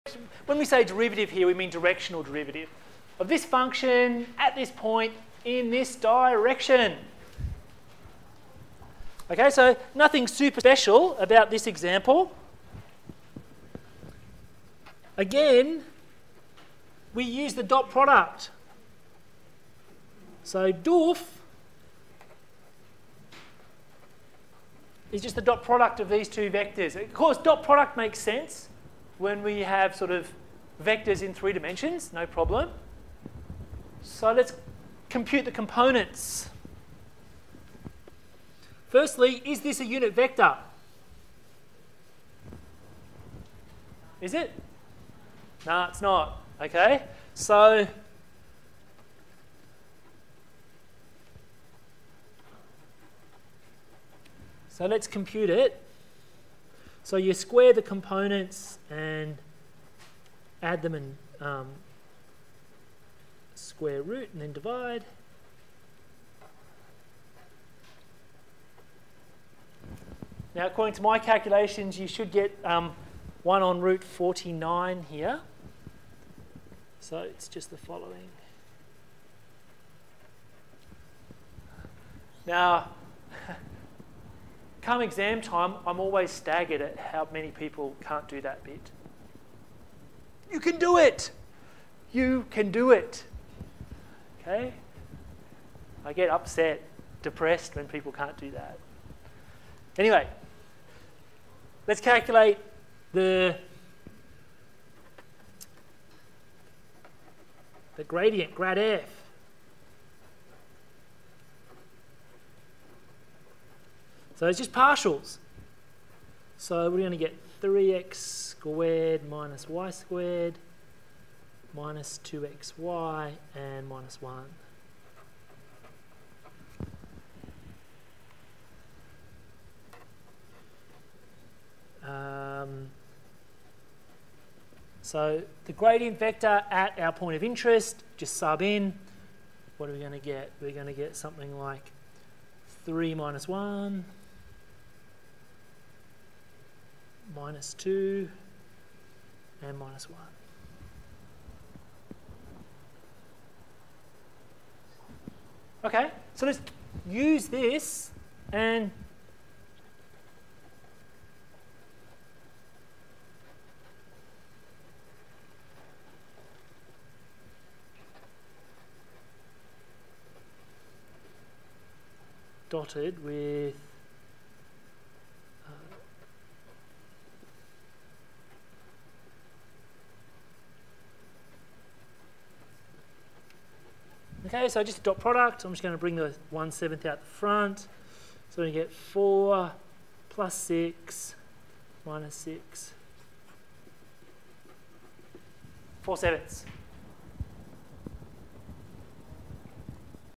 Directional derivative example
Example on directional derivative of a function of two variables.